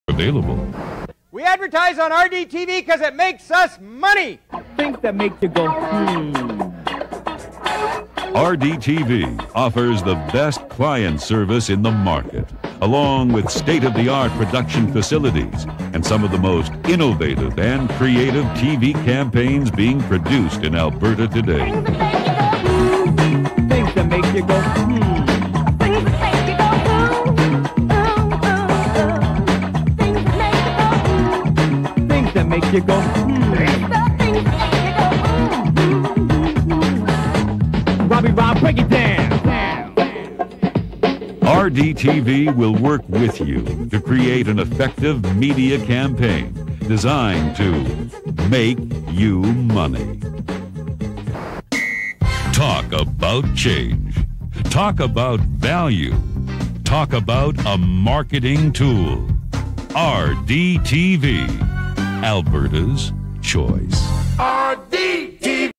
RDTV Creative services 1993 Promo video
Promo video for RDTV's Creative Service Department from the 1993 Fall Launch video.